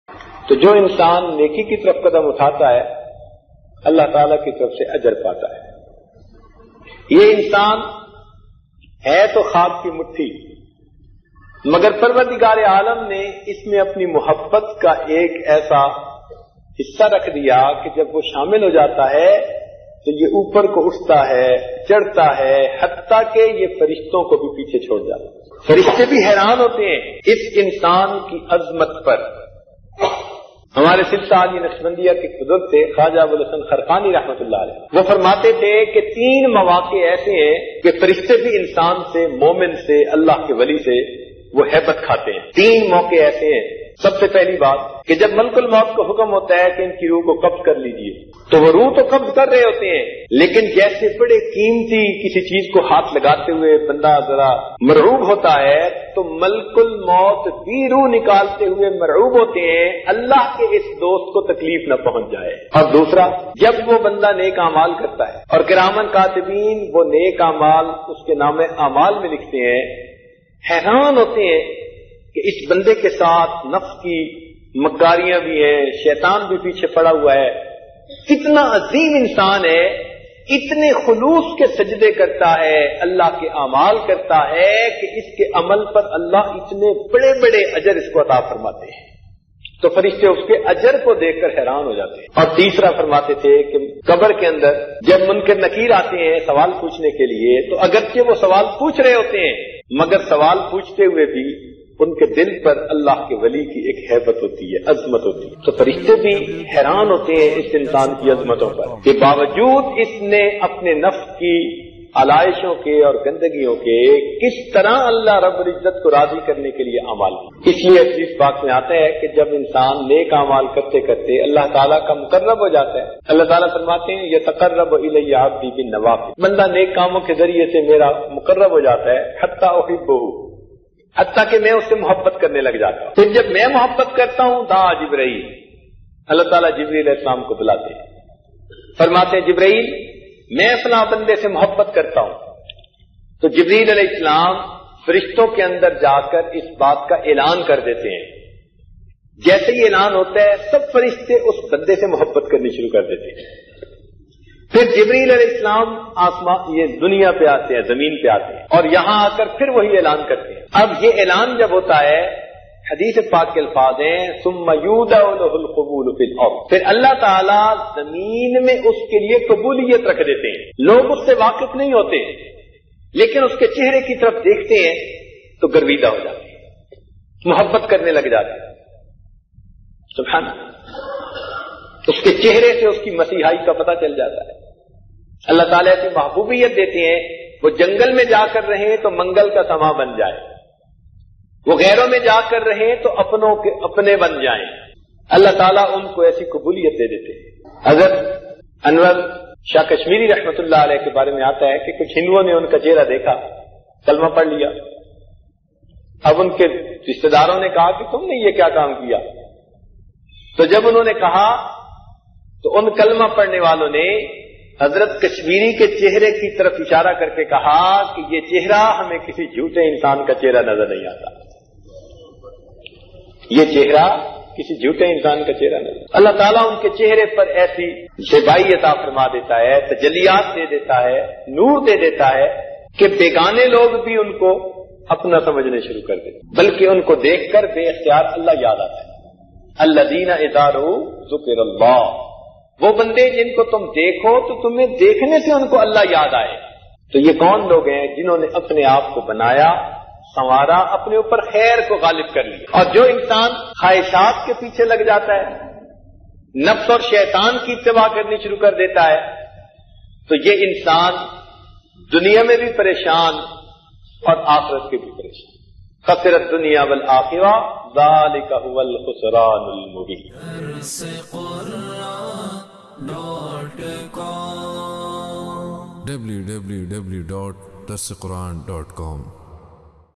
"Kis Insan Par Firishtay Rashak Kartay Hain?" Molana Peer Zulfiqar Ahmad Naqshbandi Sahab
"Kis Insan Par Firishtay Rashak Kartay Hain?"
 Molana Peer Zulfiqar Ahmad Naqshbandi Sahab
"کس انسان پر فرشتے رشک کریں گے؟"
مولانا پیر ذوالفقار احمد نقشبندی صاحب